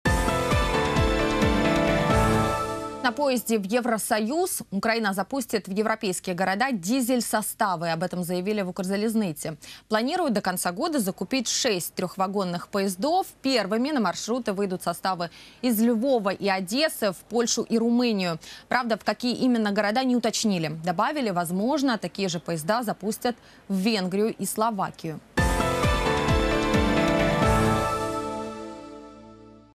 На поезде в Евросоюз
Украина запустит в европейские города дизель-составы. Об этом заявили в Укрзализныце. Планируют до конца года закупить шесть трёх-вагонных поездов. Первыми на маршруты выйдут составы из Львова и Одессы - в Польшу и Румынию. Правда, в какие именно города – не уточнили. Добавили - возможно, такие же поезда запустят в Венгрию и Словакию.